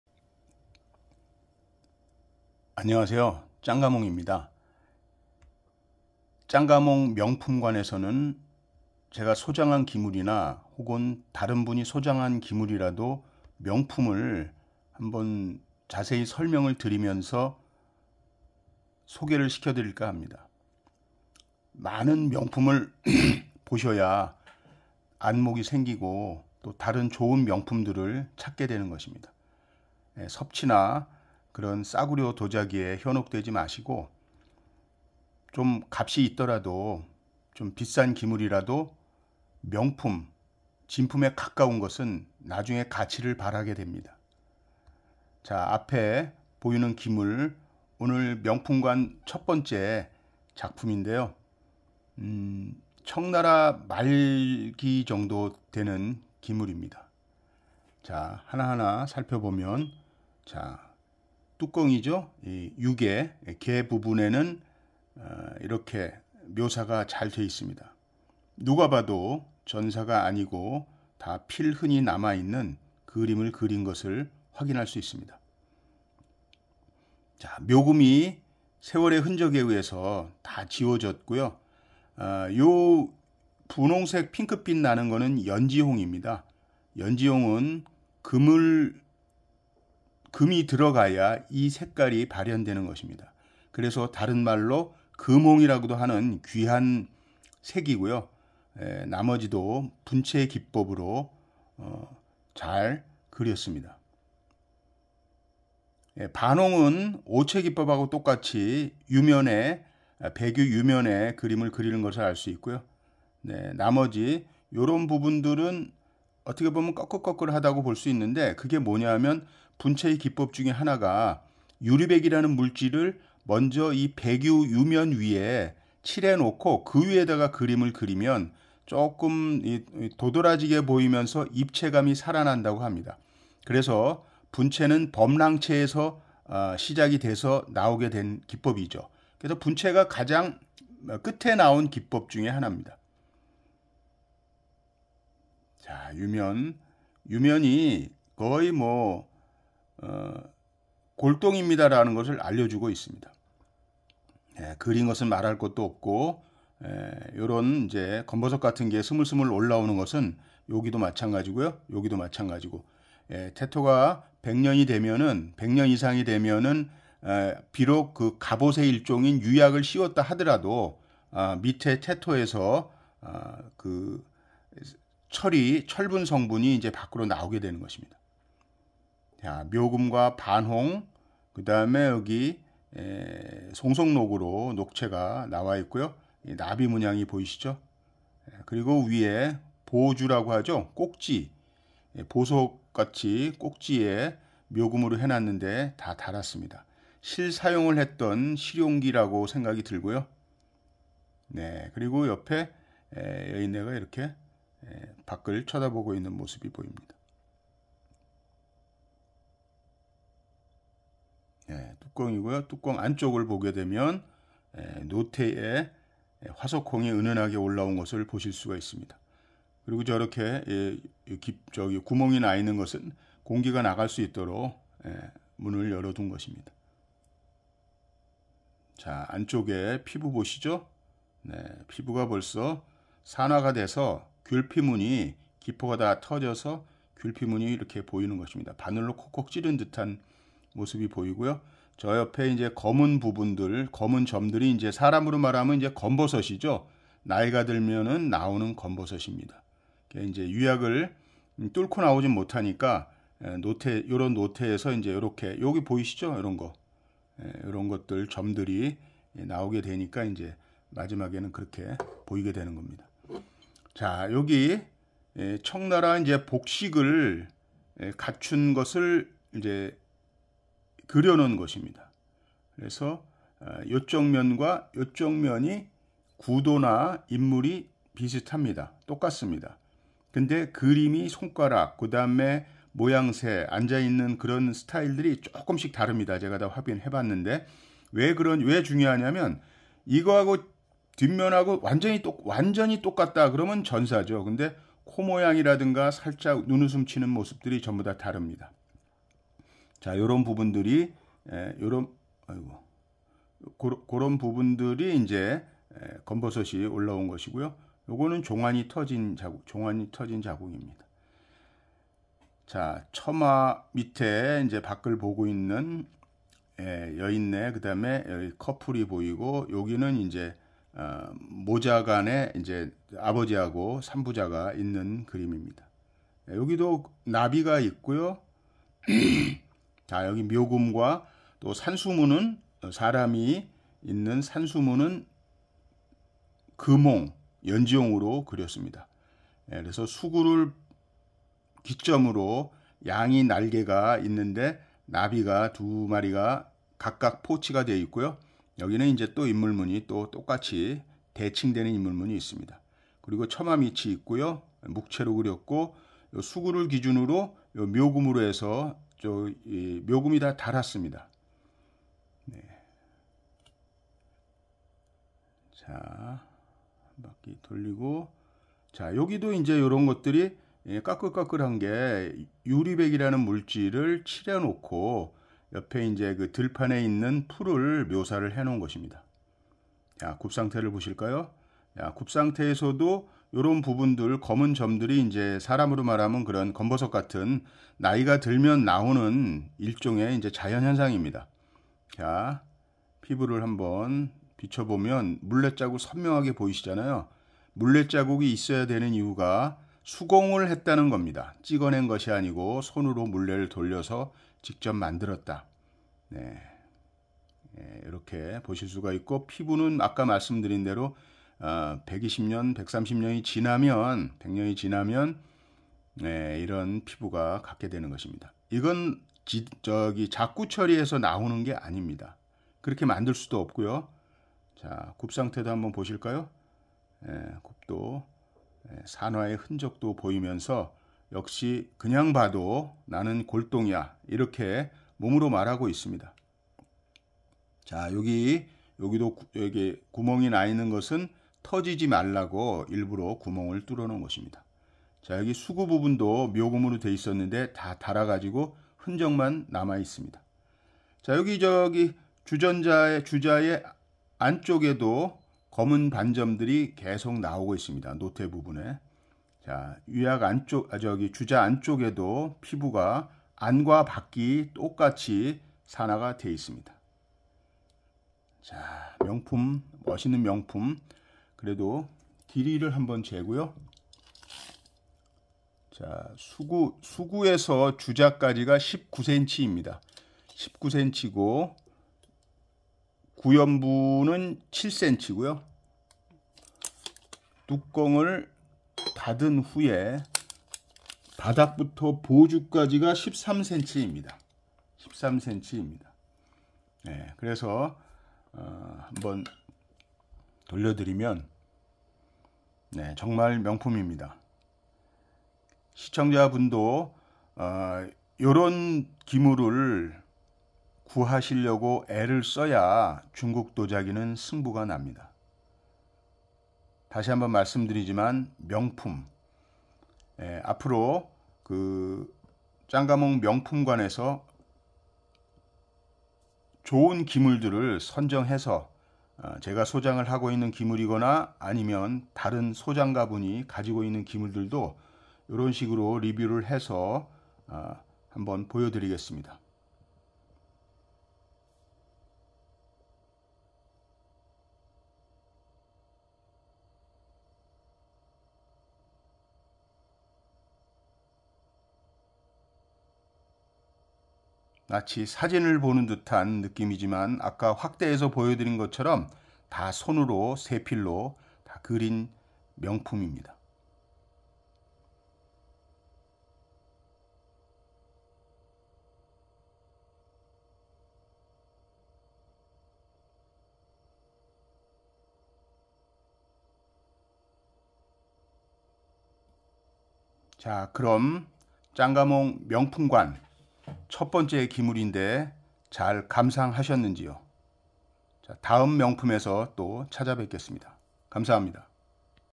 중국도자기진품명품관/1ㅡ분채청복식인물문집호ㅡ청말골동ᆢ높이13직경19/(01073388729)/짱가몽옥션/매일오후9시30분/짱가몽도썰/구독감사합니다^^
@중국도자기진품ㆍ명품관ㅡ소장가 명품 감상
진품ᆢ그 시대 기물이거나 진귀한 기물
명품ᆢ방품이지만 진품에 가까운 기물